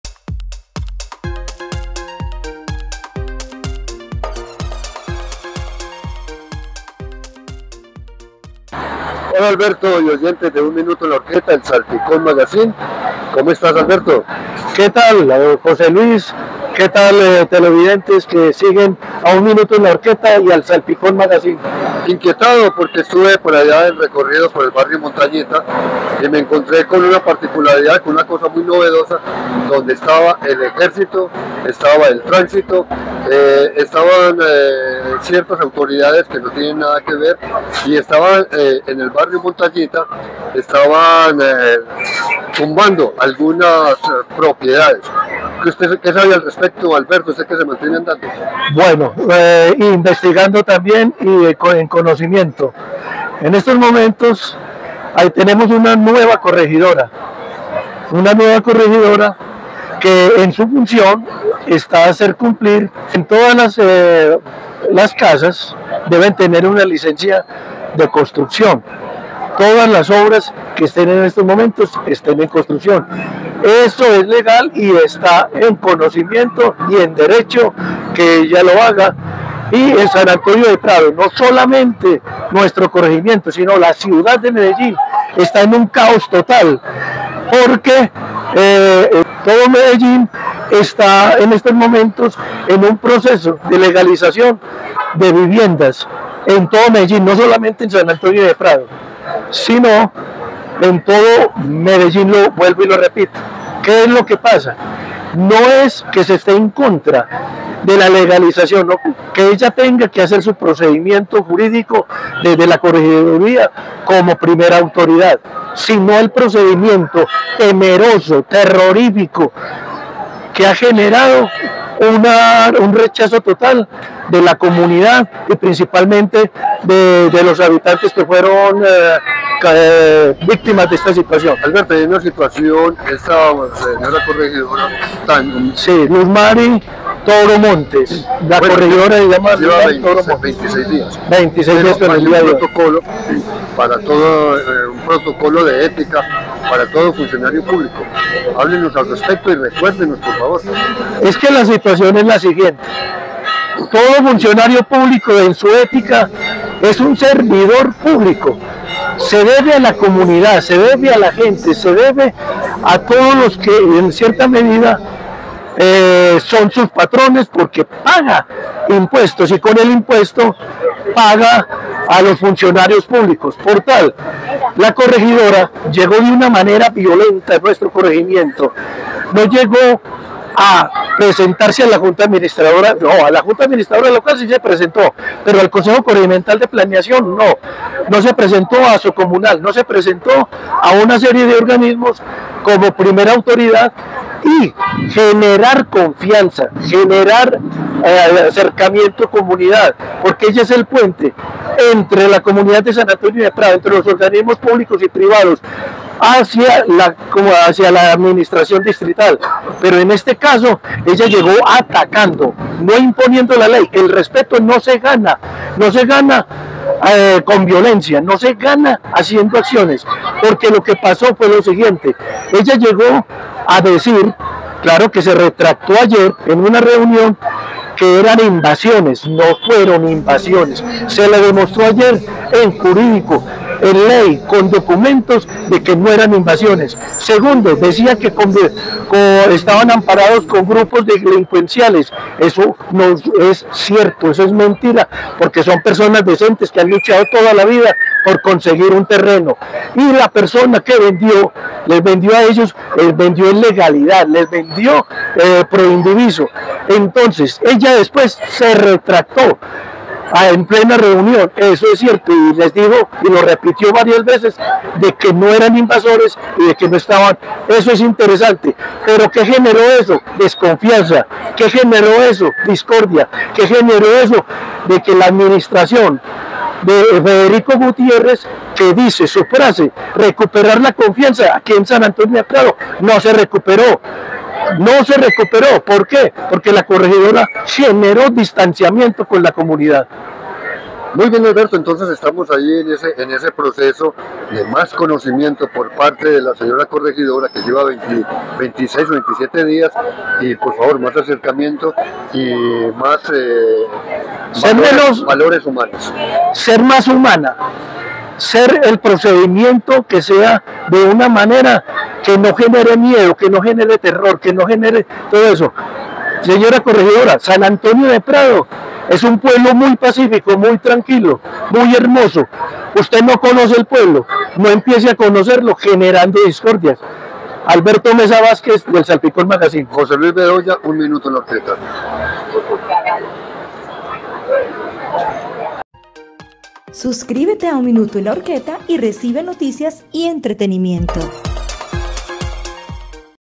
"Comunidad en alerta: La corregidora debe cambiar su actitud"
Malestar  generó la nueva corregidora de la comuna 80 Luz Mary toro Montes;
 la comunidad le pide cambio de actitud"